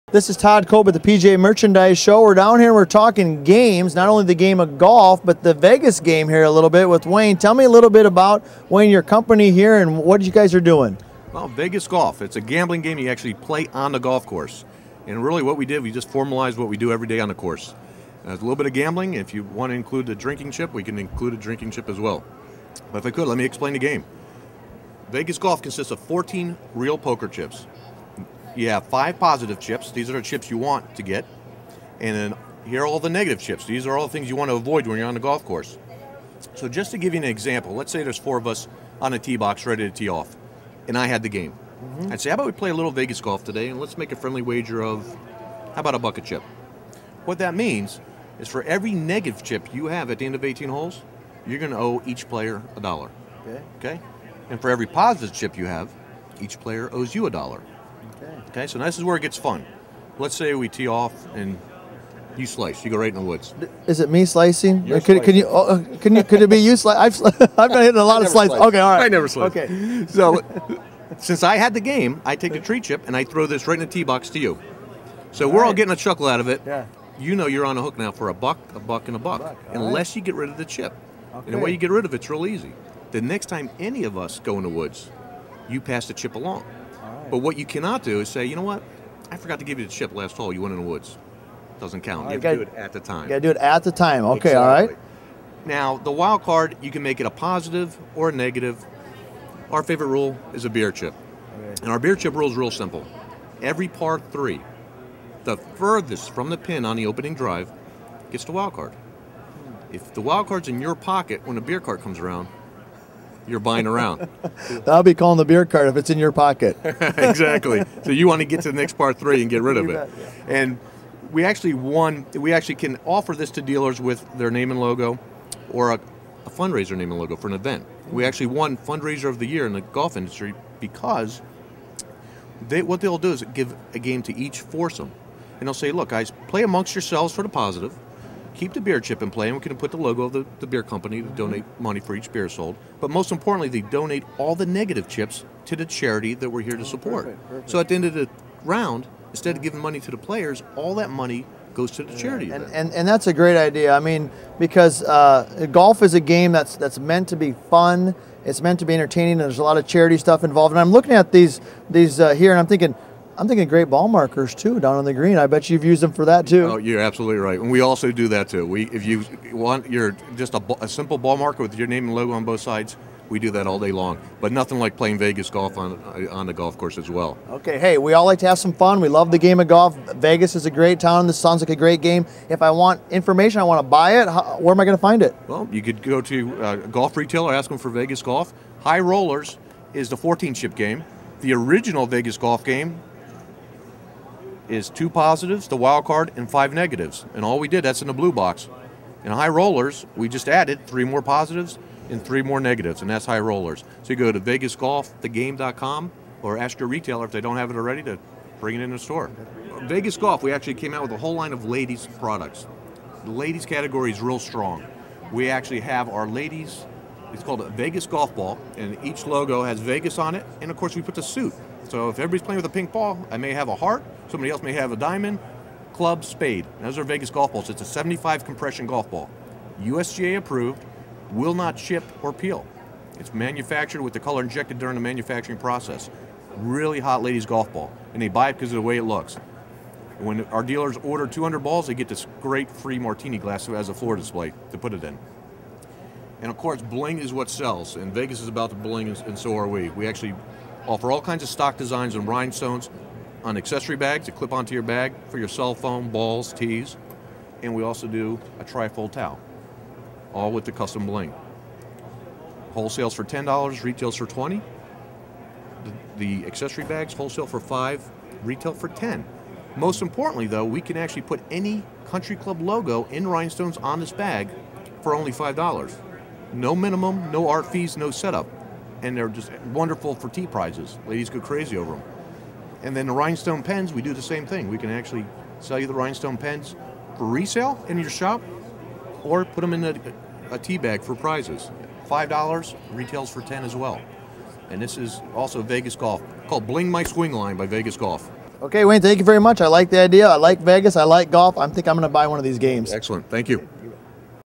Vegas Golf The Game
Vegas Golf the Game - the fun game you play with poker chips WHILE playing golf!
Rhinestone Ladies Golf Accessories from FOXYWARE